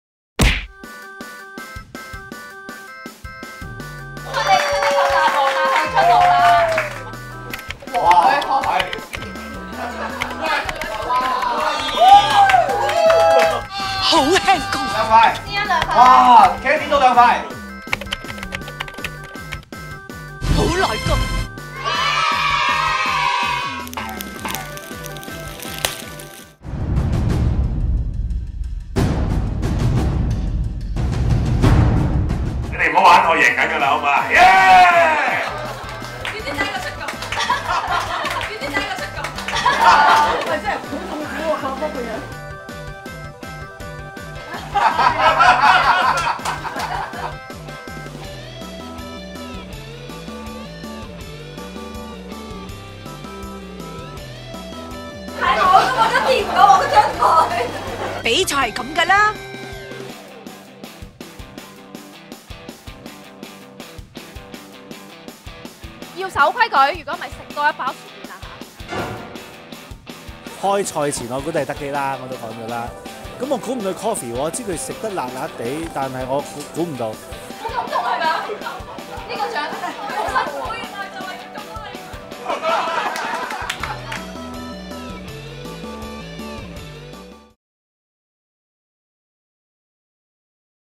【挑辣星極辣挑戰賽】辣王辣后必殺技
經過四個回合搵命搏，伴隨住一連串汗水加淚水，辣王辣后出爐， 想知辣王黃德几，及辣后林芊妤(Coffee) 有咩食辣必殺技，同埋其他參賽者點樣搏命挑戰，即刻去片！

睇完四個回合，夠膽嘅歡迎跟住挑戰！